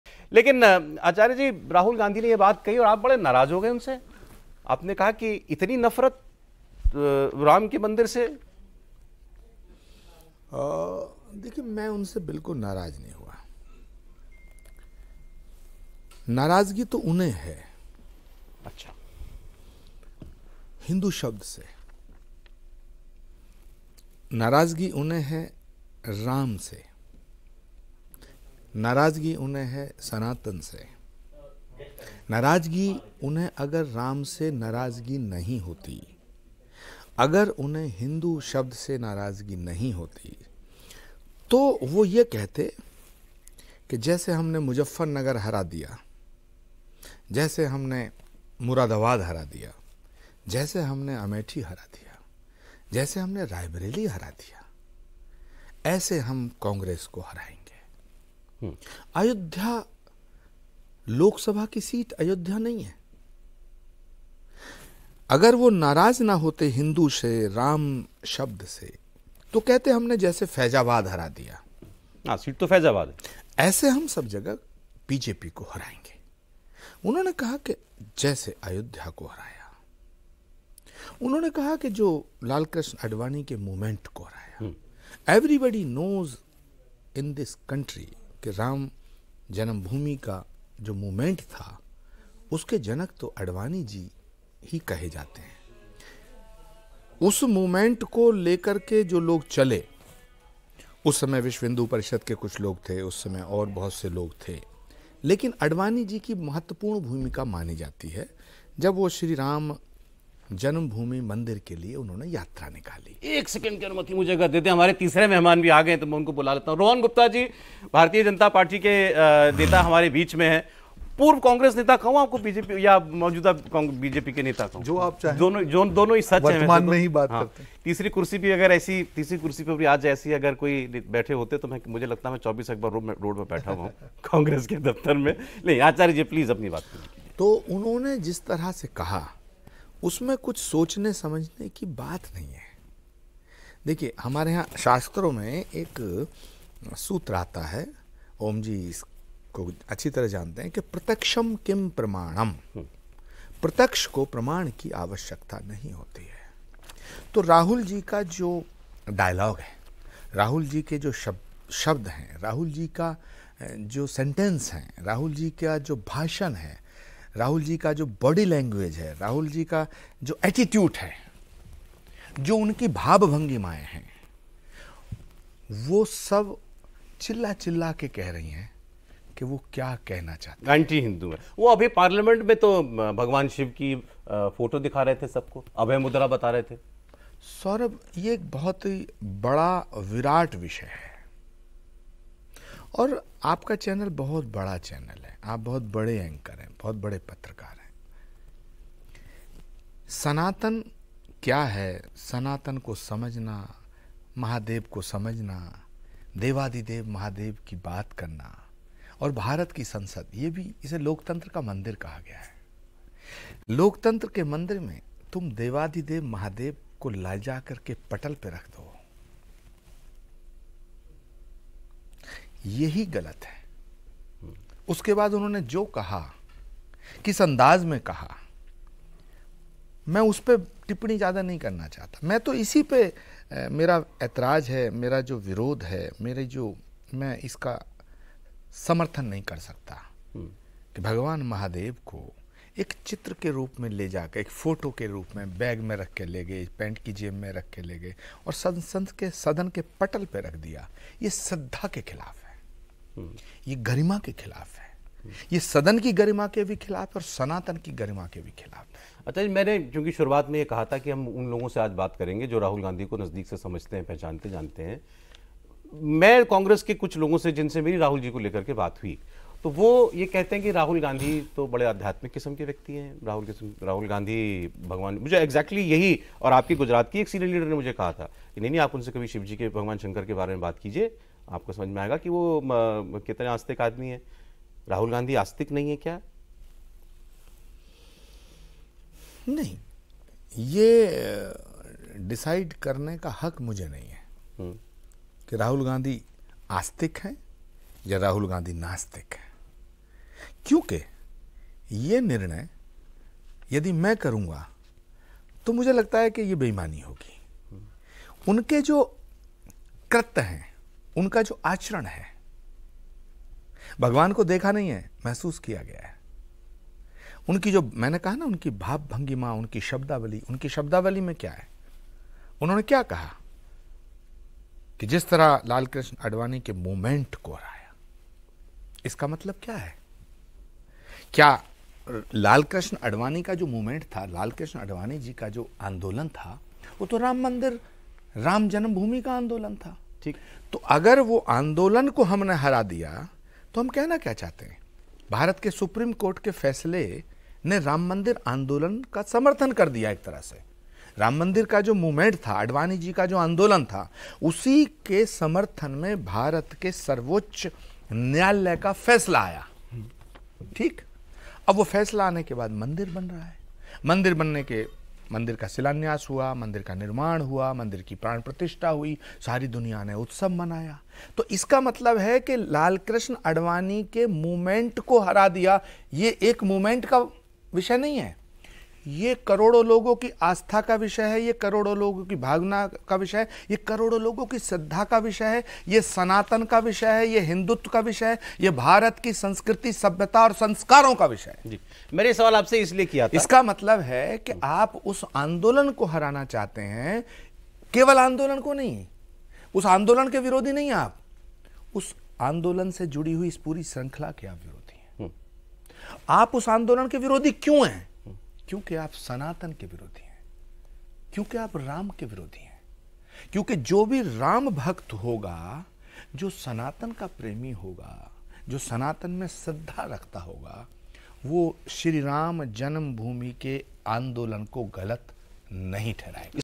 Pramod Krishnam On Rahul Gandhi: प्रमोद कृष्णम क्यों राहुल गांधी से नाराज हैं?...सुनें | PM Modi
Gujarat Assembly Election News: गुजरात में बीजेपी को कैसे हराएगी कांग्रेस?...गुजरात के लिए राहुल का अयोध्या का सहारा क्यों?...मोदी के गढ़ में राहुल आसानी से जीत जाएंगे ?